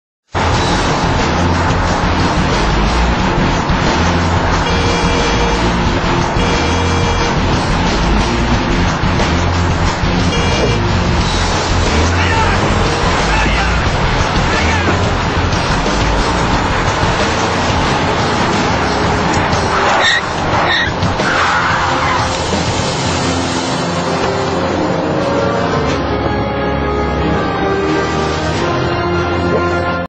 How to keep an aggressive driver at a safe distance
Just show them your little gun

Yes we know its fake people, don't try this at home